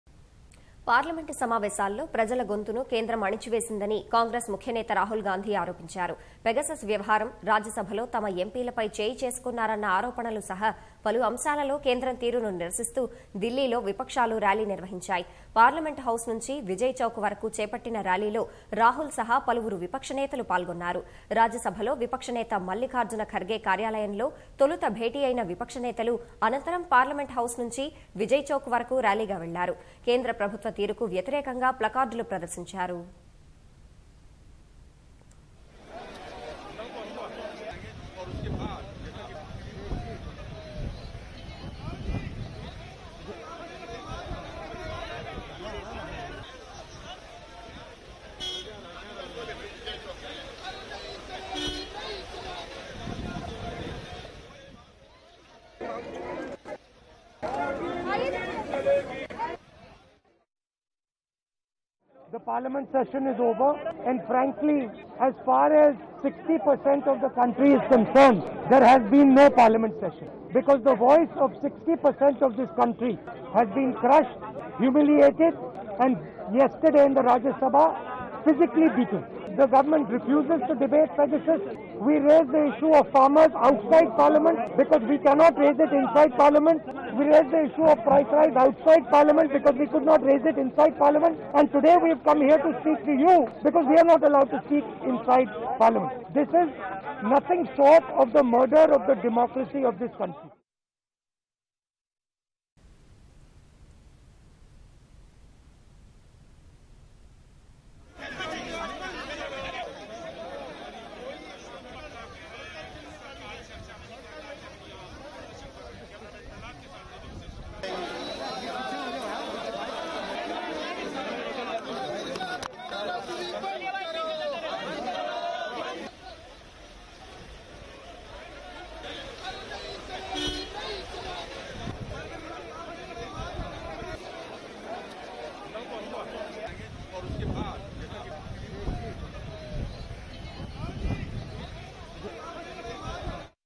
కేంద్రం తీరు నిరసిస్తూ ప్రతిపక్షాల ర్యాలీ | Opposition Rally in Delhi | Repeal of Agricultural Laws
పార్లమెంట్ సమావేశాల్లో ప్రజలగొంతును కేంద్రం అణిచివేసిందని కాంగ్రెస్ ముఖ్యనేత రాహుల్ గాంధీ. ఆరోపించారు. పెగసస్ వ్యవహారం, రాజ్యసభలో తమ ఎంపీలపై చేయి చేసుకున్నారన్న ఆరోపణలు సహా. పలు అంశాలలో కేంద్రం తీరును నిరసిస్తూ.. దిల్లీలో విపక్షాలు ర్యాలీ నిర్వహించాయి. పార్లమెంట్ హౌస్ నుంచి విజయ్ చౌక్ వరకూ చేపట్టిన ర్యాలీలో రాహుల్ సహా పలువురు విపక్షనేతలు పాల్గొన్నారు. రాజ్యసభలో విపక్ష నేత మల్లిఖార్జున ఖర్గే కార్యాలయంలో తొలుత భేటీయైన విపక్షనేతలు అనంతరం పార్లమెంట్ హౌస్ నుంచి విజయ్ చౌక్ వరకూ... ర్యాలీగా వెళ్లారు. కేంద్ర ప్రభుత్వ తీరుకు వ్యతిరేకంగా ప్లకార్డులు ప్రదర్శించారు.